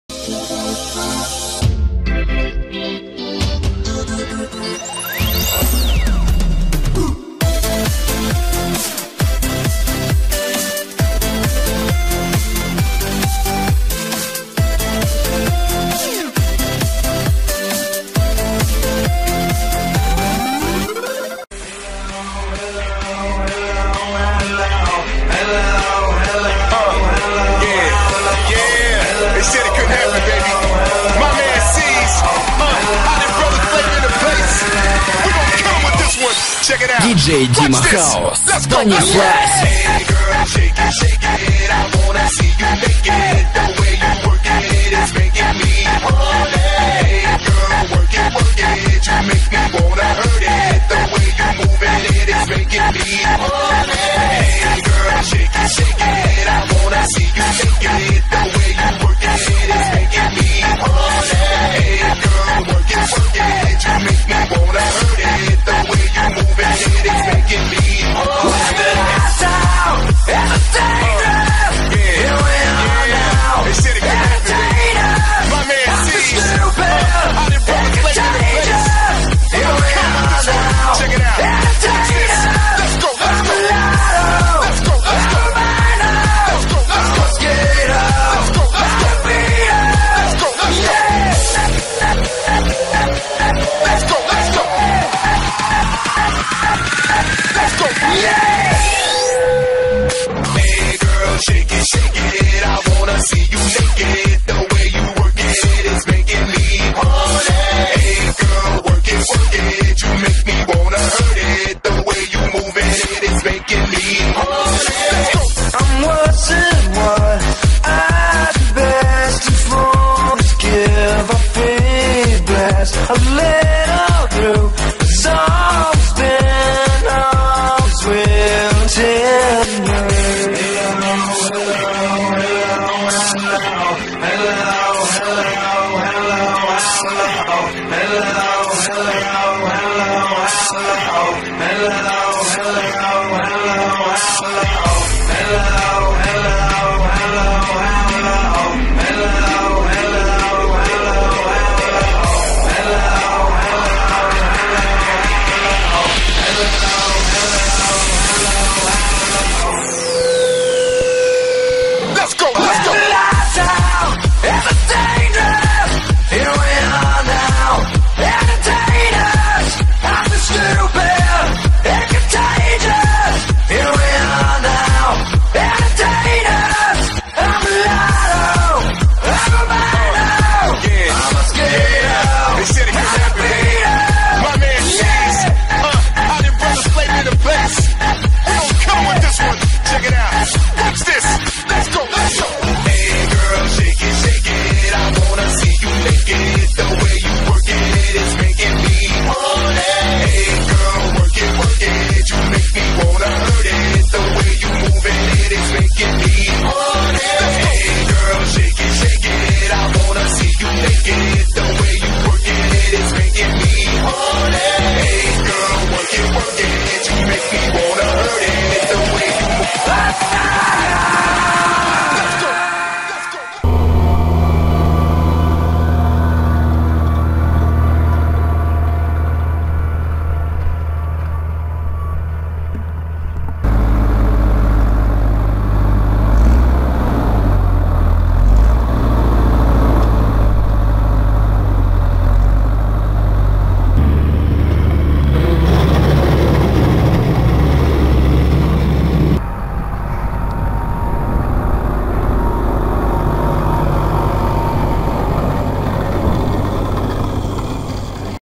㋡Mały Mix✭ Gruberowanie + Wywóz obornika ㋡
Siemano dzisaj macie Mały Mix Gruberowanie + Wywóz obornika  Bo nie wiedziałem czy wzuci osobno czy razem i wyszło take coś xd :D
Życzę miłego oglądania d(-_-)b

☆ Wystąpili :
• Steyr Profi 6140
• Steyr 9085MT
•Zetor ZTS 16245
•Zetor 7745